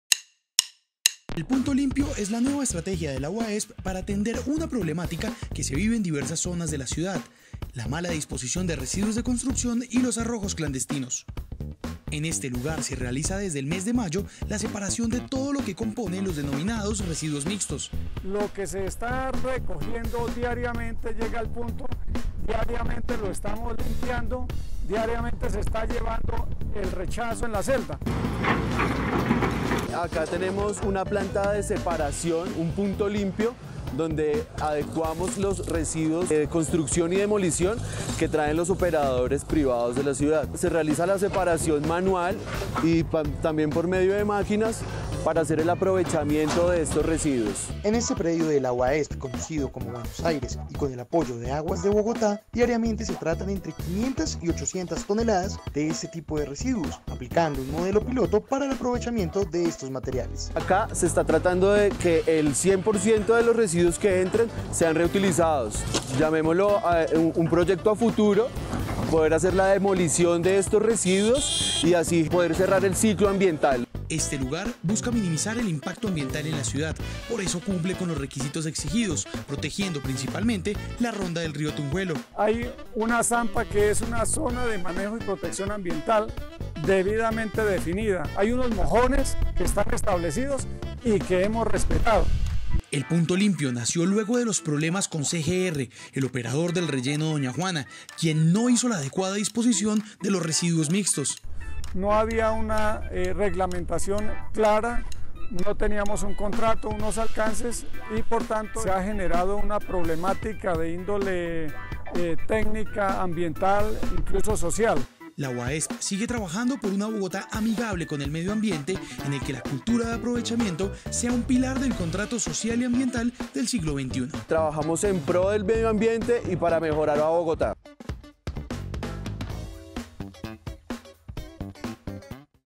Bogotá estrena un "PuntoLimpio" para el aprovechamiento de residuos abandonados
Con la estrategia se busca atender miles de toneladas de residuos que son abandonados en las calles de la ciudad cada día.